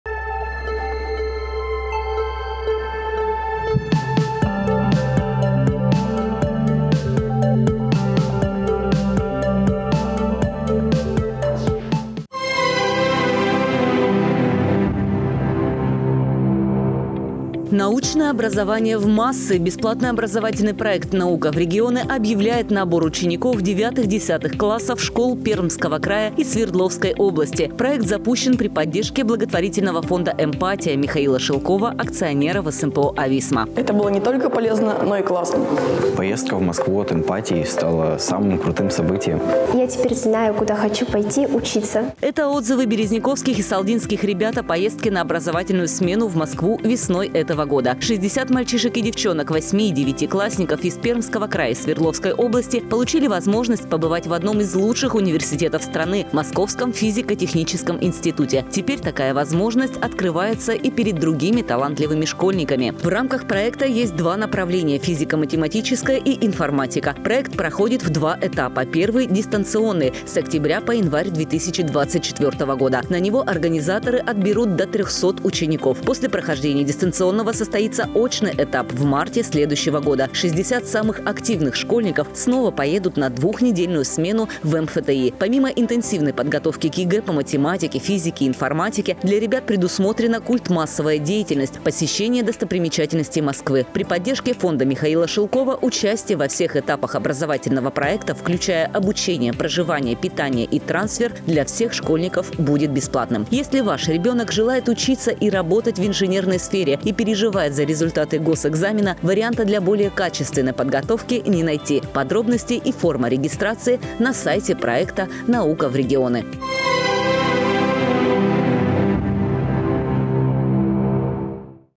Проект "Наука в регионы" объявляет набор школьников
Научное образование в массы! Бесплатный образовательный проект «Наука в регионы» объявляет набор учеников 9-10 классов школ Пермского края и Свердловской области. Проект запущен при поддержке Благотворительного фонда «Эмпатия» Михаила Шелкова, акционера ВСМПО-АВИСМА.